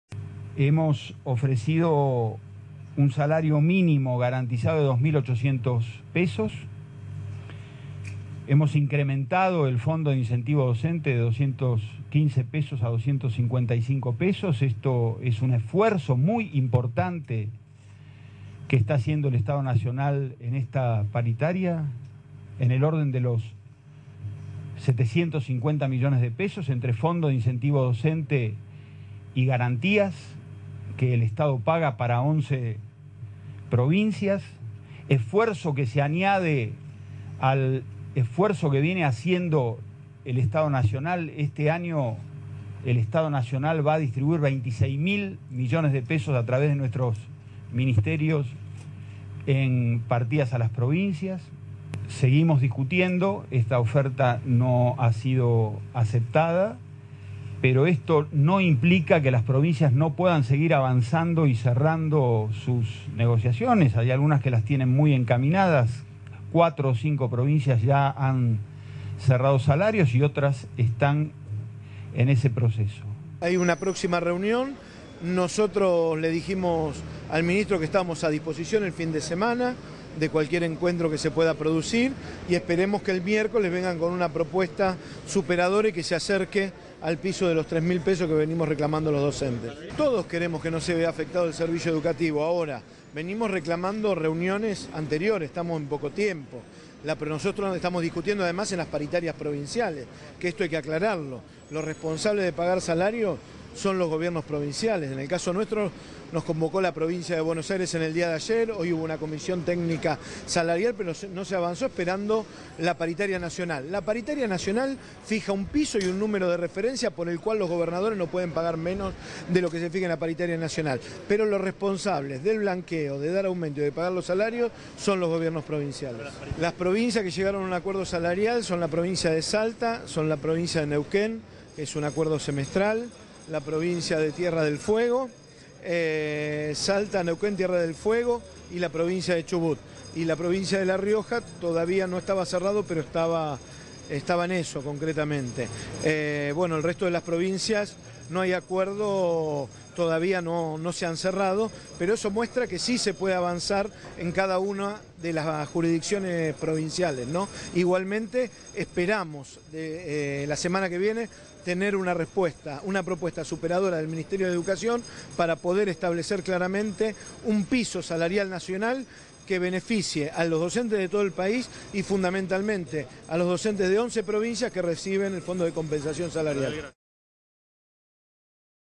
Sindicatos no aceptan nueva propuesta salarial
Los cinco sindicatos docentes con representación nacional no aceptaron hoy la nueva propuesta de piso mínimo salarial ofrecida por el Ministerio de Educación de la Nación, que consistió en 2.800 pesos de sueldo inicial y 255 pesos para el Fondo de Incentivo Docente.

Los representantes gremiales acordaron una nueva reunión para el miércoles próximo ya que mantuvieron su reclamo de 3.000 pesos de salario inicial, equivalente a un incremento del 28% respecto al año pasado.

"La oferta que acabamos de exponer es de 2.800 pesos de salario inicial y elevar de 215 a 255 pesos el incentivo docente, lo que demanda unos 750 millones de pesos de inversión del Estado Nacional en el salario de los maestros, entre incentivos y garantías para 11 provincias", dijo Sileoni.

El ministro destacó que "esta oferta no ha sido aceptada, seguimos discutiendo, pero esto no implica que las provincias sigan avanzando en sus negociaciones, ya hay cuatro o cinco que las tienen muy encaminadas".

"En materia educativa hemos cambiado mucho en los últimos años, los derechos de los trabajadores fueron compensados y resguardados en este último tiempo, pero no concebimos que haya derecho que este por sobre el derecho de los chicos a recibir clases", enfatizó el ministro.

En este sentido manifestó que "seguimos con el espíritu de diálogo pero si no se llegara a un acuerdo no hay razones para no brindar el servicio educativo".

Mencionó además entre los esfuerzos que realiza el Estado a la distribución de "26 mil millones de pesos en partidas a las provincias para la educación".

Estela Maldonado, de la Confederación de Trabajadores de la Educación de la República Argentina (CTERA), dijo a Télam que "hemos rechazado unánimemente todos los sindicatos los 2.800 para el salario inicial del maestro de grado de jornada simple sin antigüedad".

Indicó que "hay un cuarto intermedio hasta el miércoles 22 de febrero en horario a determinar, porque la negociación ha quedado abierta".

Precisó que rechazaron la nueva propuesta "por considerarla insuficiente, ya que no se acerca a nuestra demanda que tiene 3.000 pesos para el salario inicial".

Maldonado destacó que aunque a alguno le pueda parecer que es menor la diferencia pero respecto a un salario tan bajo como nuestro inicial, que es de 2.340 pesos, sigue siendo un monto importante".

"En realidad, como para llegar a ese monto inicial hay cifras que completan, achatando las pirámides salariales -como el incentivo y otras cifras que ponen las provincias- resulta que hasta los siete años de antigüedad, prácticamente todo el mundo gana eso o apenas 50 pesos más".

Sileoni sostuvo que el gobierno nacional "ratifica la construcción de un salario docente digno y recuperado en los últimos años frente a un contexto internacional de despidos y rebaja de haberes".

"Esta es una paritaria distinta a la de cualquier discusión salarial, porque tiene un cometido muy específico fijar el salario mínimo garantizado como cargo testigo de los maestros de educación primaria que trabajan un turno y que no perciben antigüedad".

En la reunión participaron además de Sileoni, la viceministra de Trabajo, Noemí Rial; el secretario de Educación, Jaime Perczyk, y los ministros de Educación de las provincias de Río Negro, Buenos Aires y Entre Ríos.

En la representación sindical estuvieron dirigentes de CTERA, la Unión de Docentes Argentinos (UDA), la Asociación del Magisterio de la Enseñanza Técnica (AMET), el Sindicato Argentino de Docentes Particulares (SADOP) y de la Confederación de Educadores Argentinos (CEA).